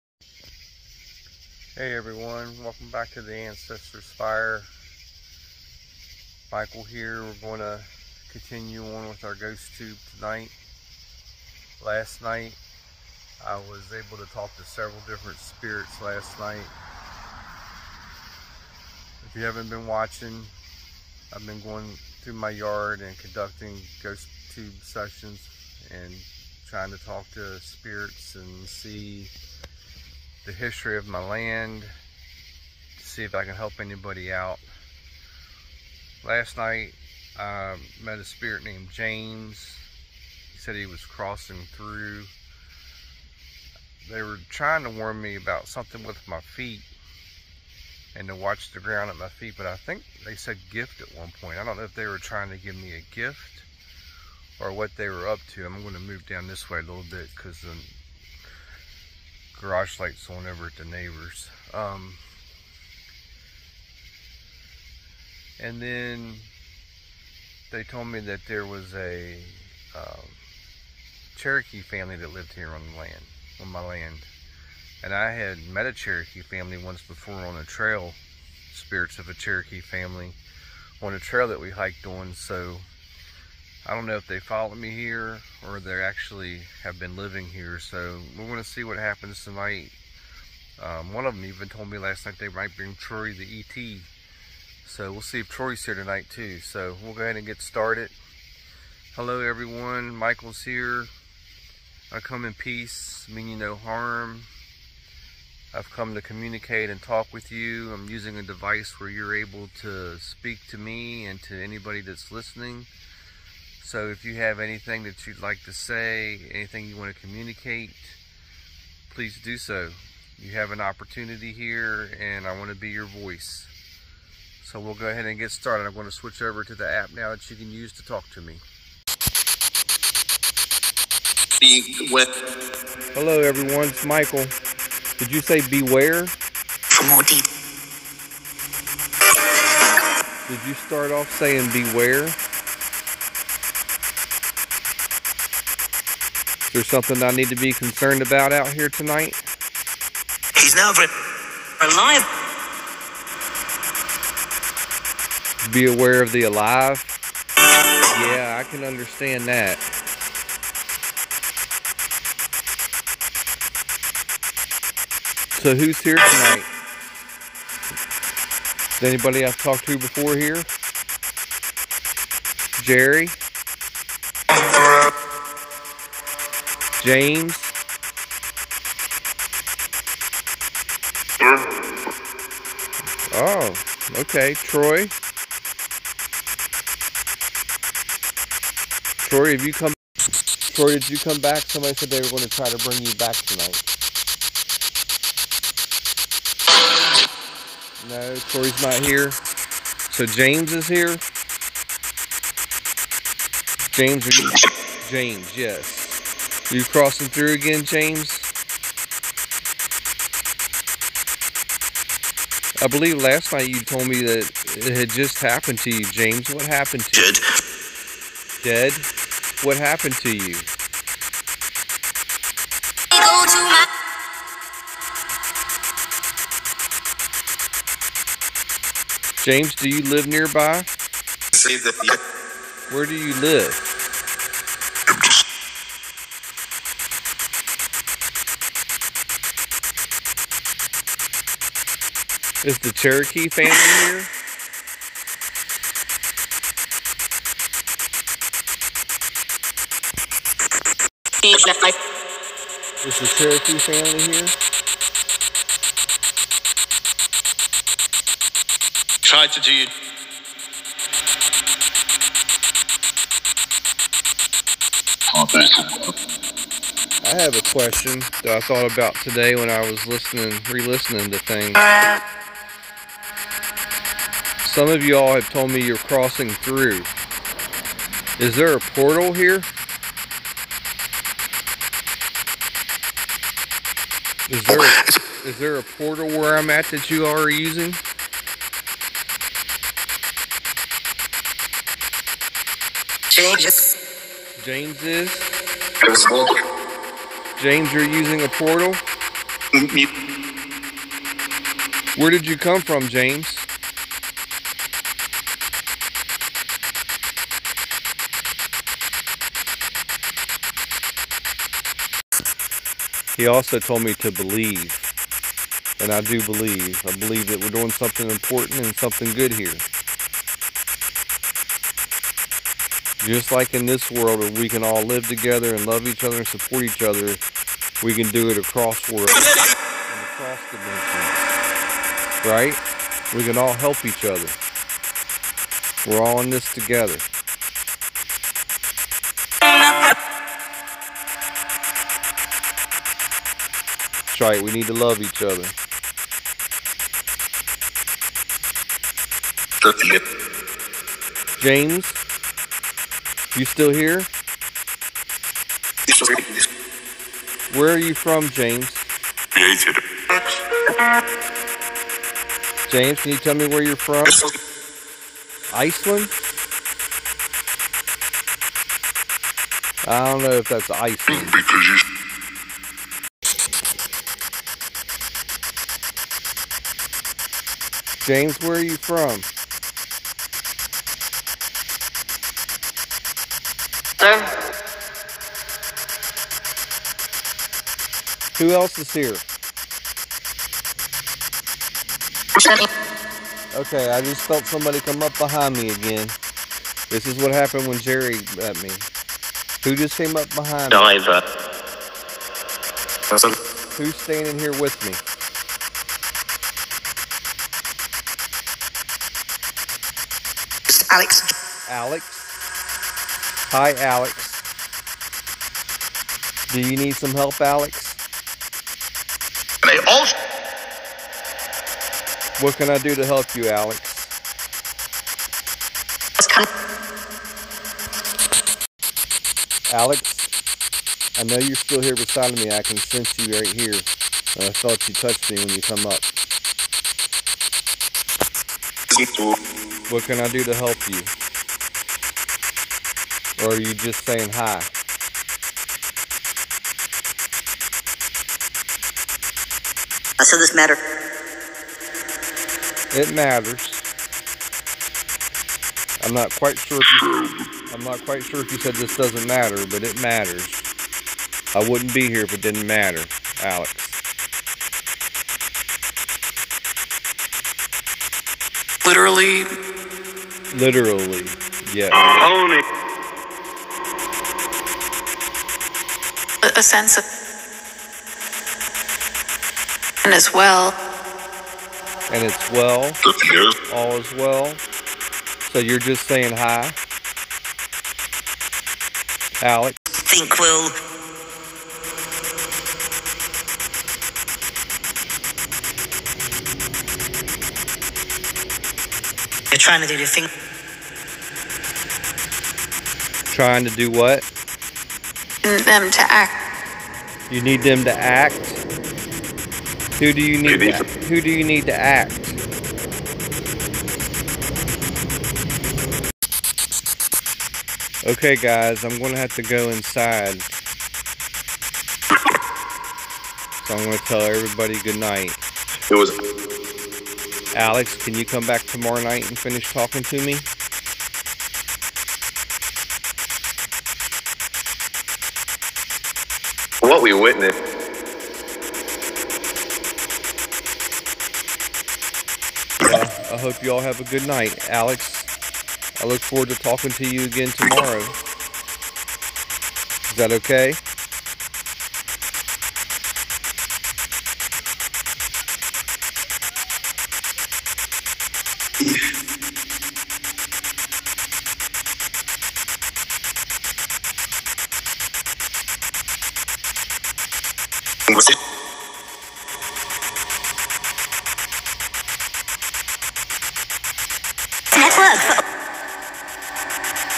Are Spirits Using A Portal On My Land?
Watch as I continue my nighttime journey through my yard to discover the history of my land and what spirits are wandering my land.

in this episode, I discuss with the spirits about the possibility of a portal on the land,
James tells me he is from Iceland, and I feel the presence of a new spirit named Alex standing next to me.